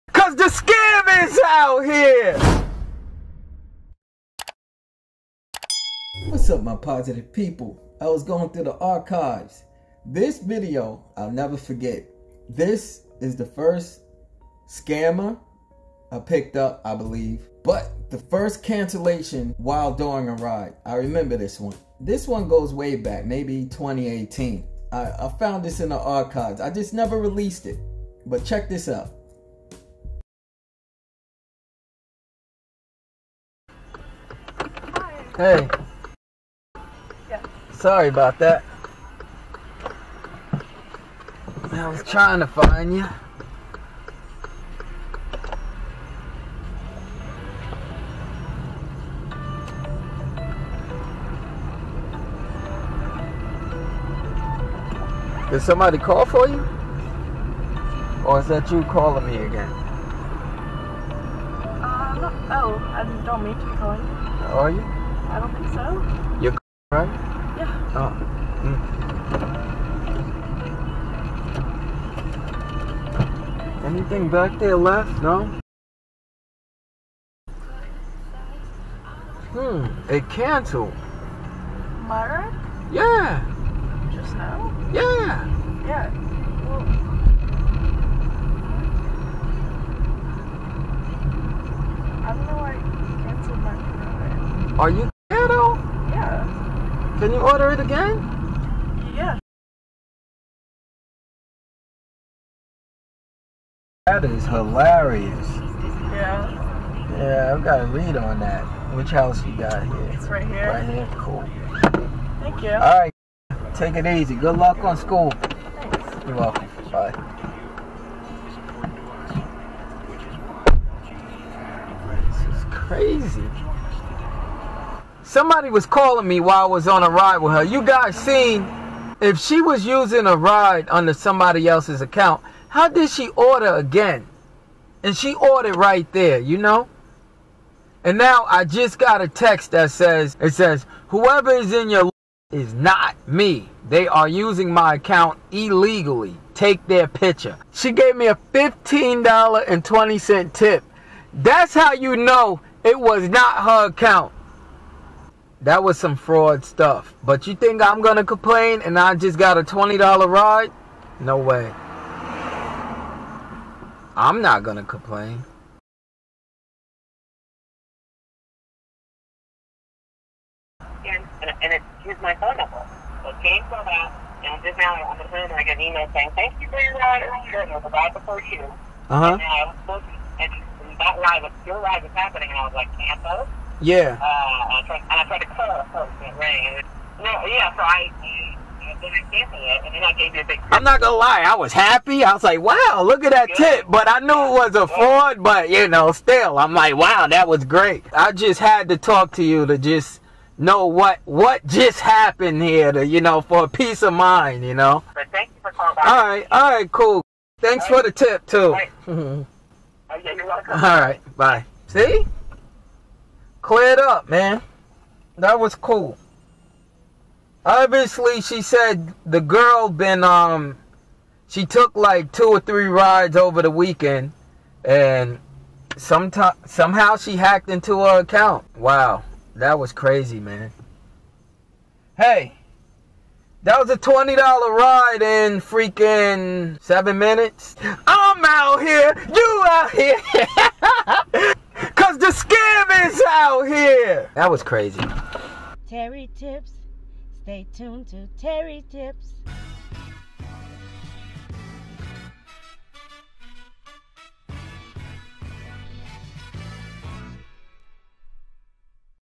My first UBER scammer & cancellation during trip
Become a DoorDash driver! Earn up to $25/hour and a bonus after deliveries in 60 days for signing up

Electric Bike used:

$terrytips